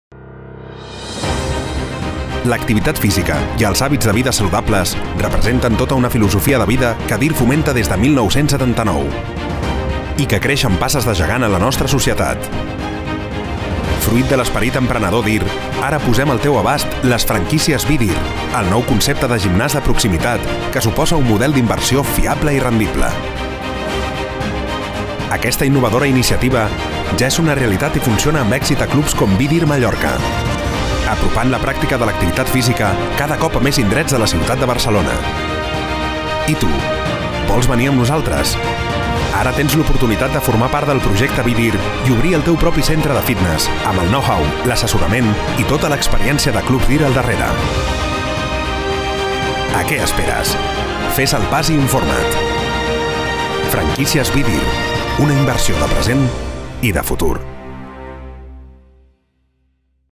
BDiR Mallorca, apropant el fitness a més indrets de Barcelona
A Clubs DiR fomentem un estil de vida saludable des de 1979, però ara fem un pas més enllà. Perquè DiR posa al teu abast les franquícies BDiR: un nou concepte de gimnàs de proximitat que s'ha convertit en un model d'inversió fiable i rendible.
Obre el teu propi club de fitness i forma part del projecte BDiR!